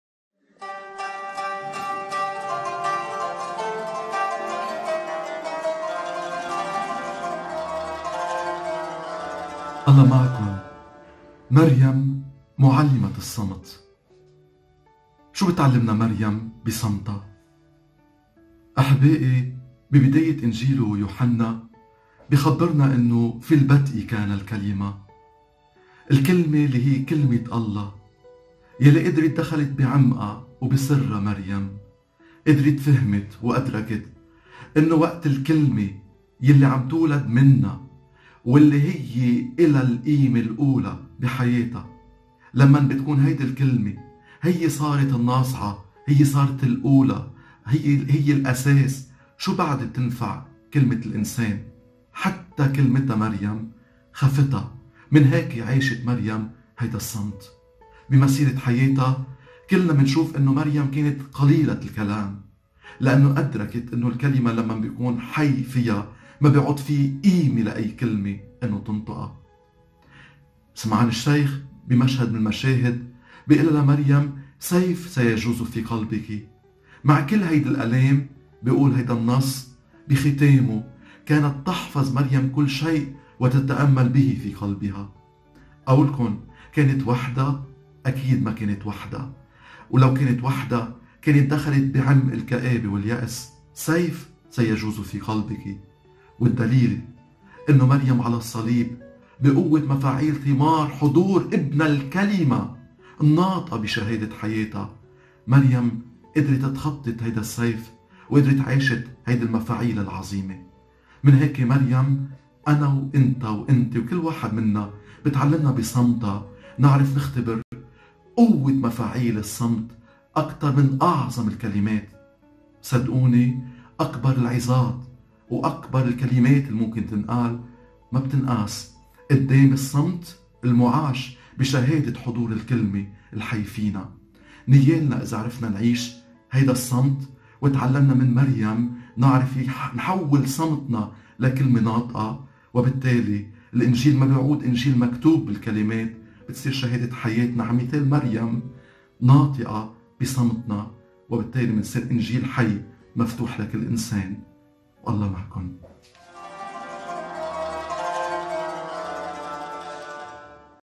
وقفة مع مريم - مريم معلمة الصمت
وقفة مع مريم
الحلقة السابع: مريم معلمة الصمت
مع الأب أندري غاوي م.ل. – رئيس اكليريكية مار يوحنا الرسول